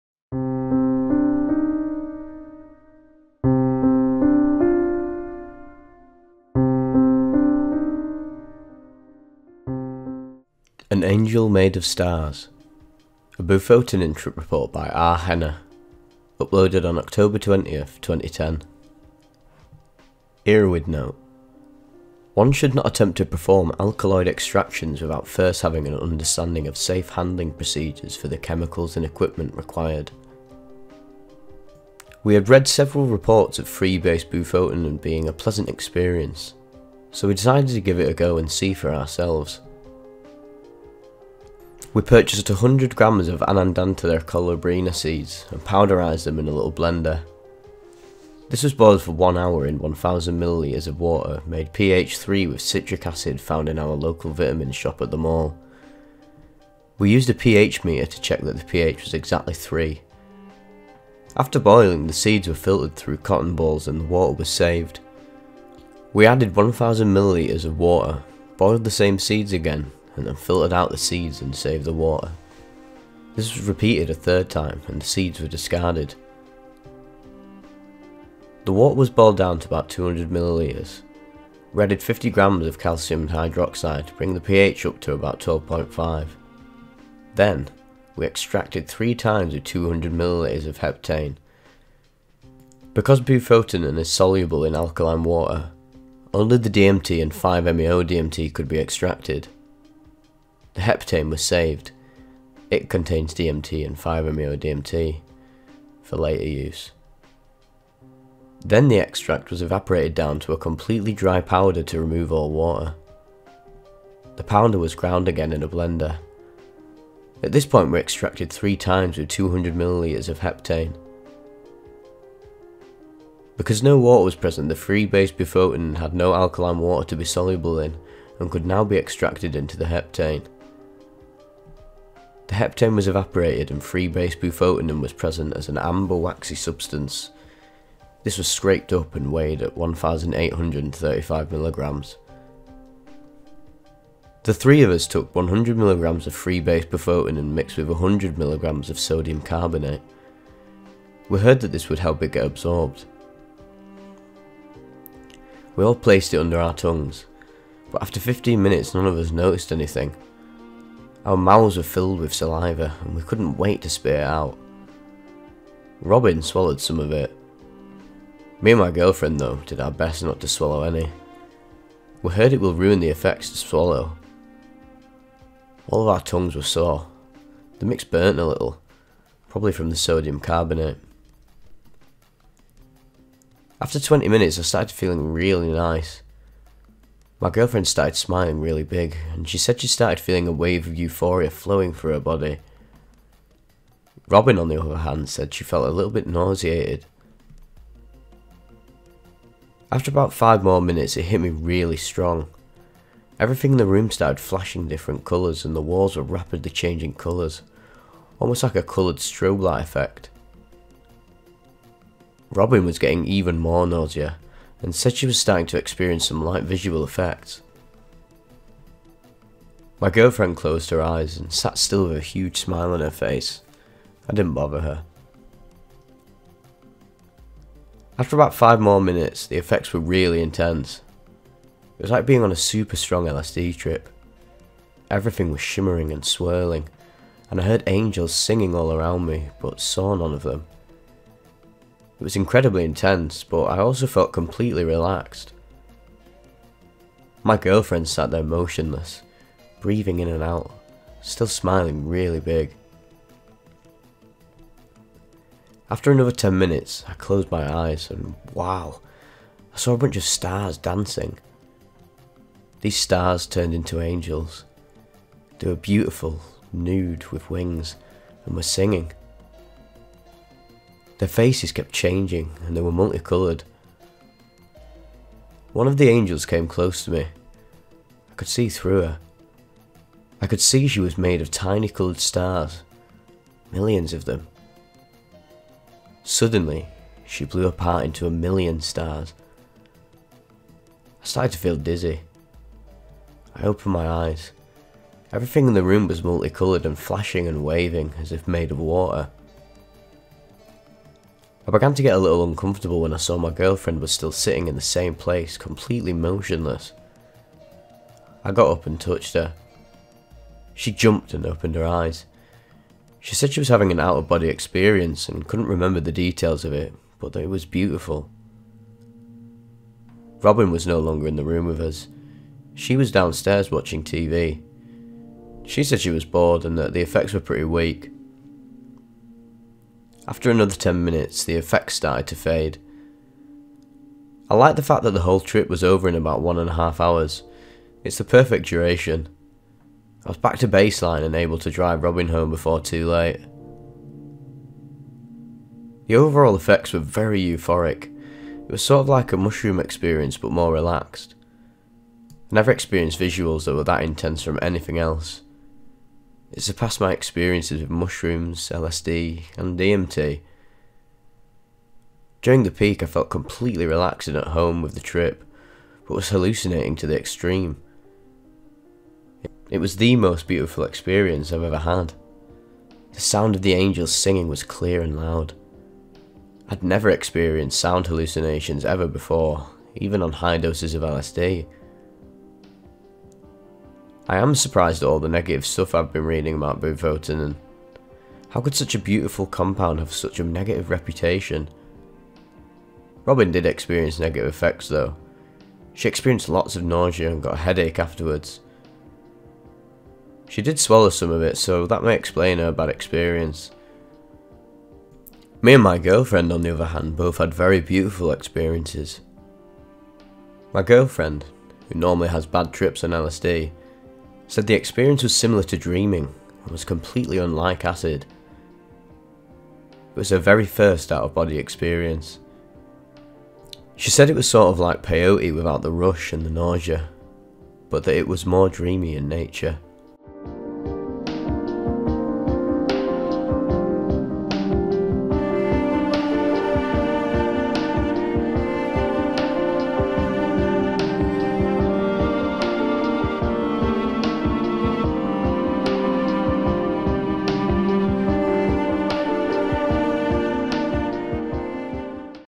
Bufotenin Trip Report: 'An Angel Made of Stars'
*THIS VIDEO DOES NOT ADVOCATE ANY OF THE FOLLOWING BEHAVIOUR AND IS INTENDED TO EDUCATE PEOPLE ON SUBSTANCE USE & HEIGHTENED STATES OF CONSCIOUSNESS. DO NOT ATTEMPT TO RECREATE ANYTHING DISCUSSED IN THE VIDEO*

Today we look at some of the effects that bufotenin can induce. It is famously associated with 'licking toads', as the bufo alvarius toad excretes the liquid that it is extracted from.